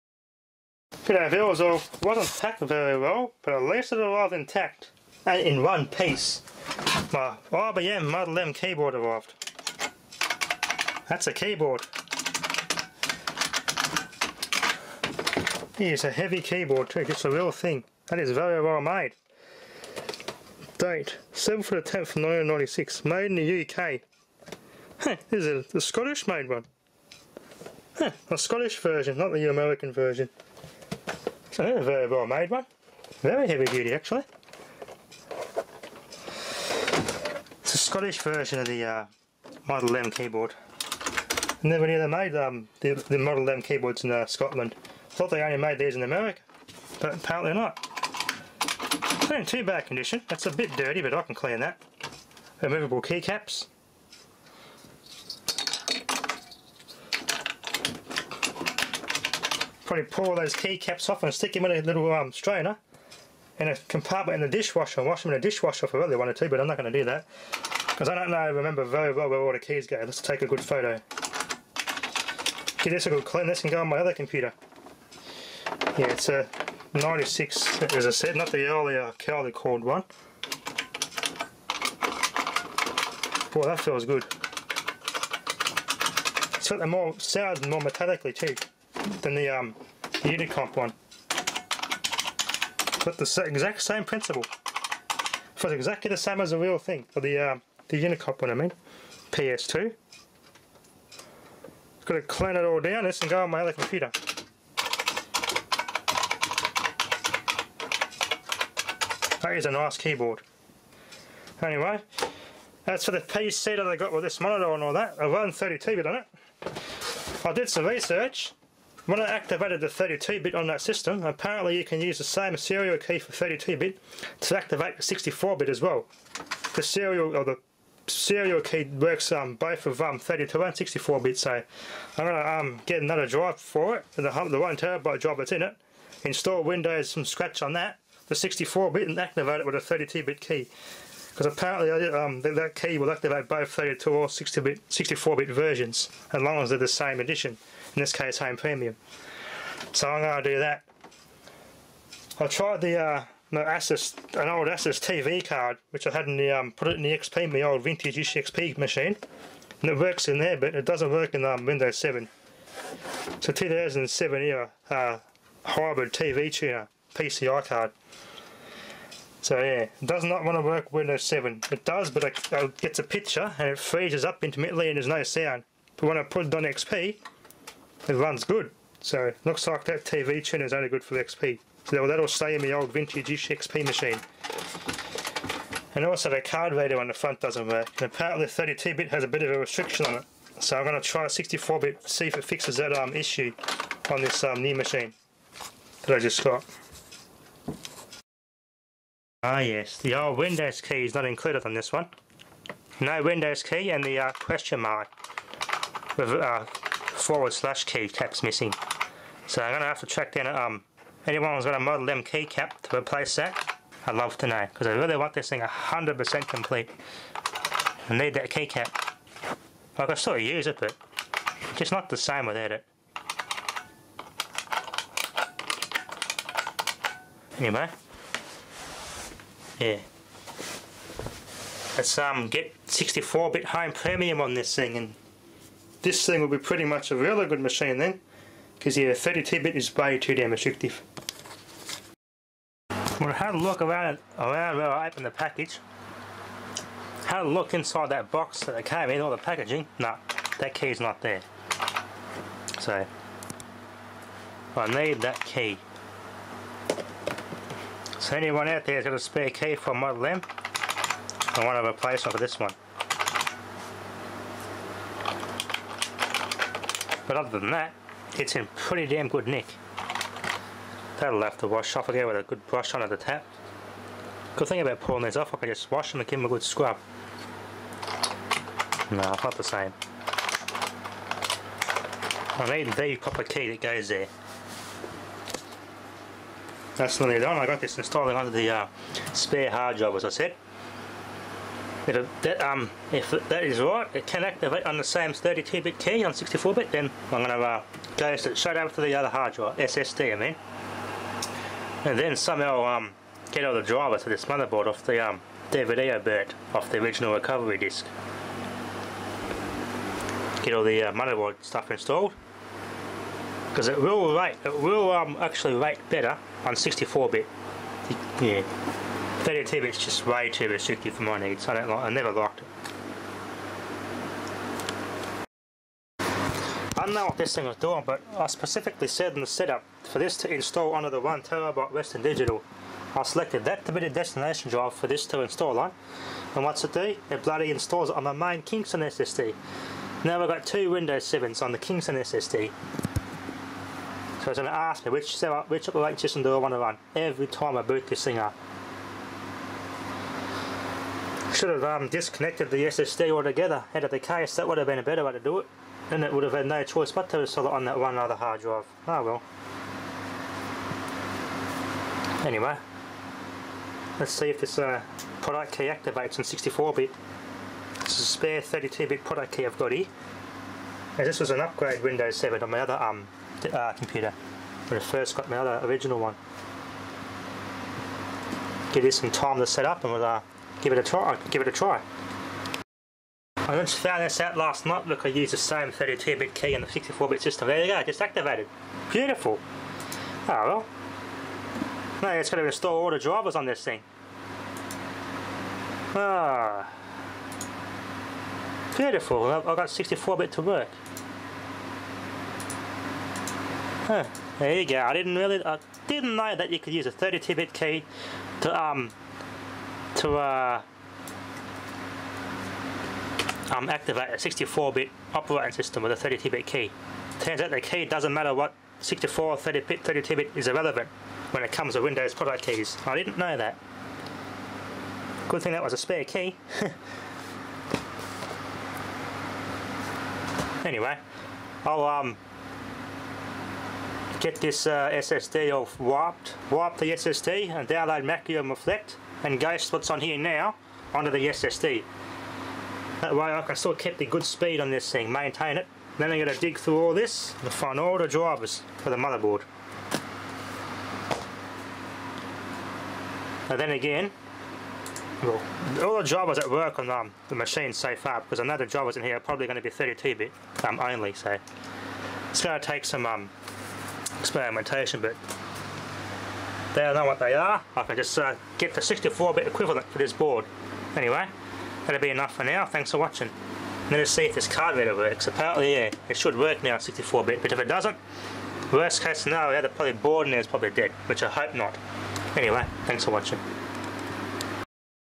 IBM Model M Keyboard & 64 Bit Win 7!!
Turns out 32 bit & 64 Bit Keys are the same!! & awesome IBM keyboard score!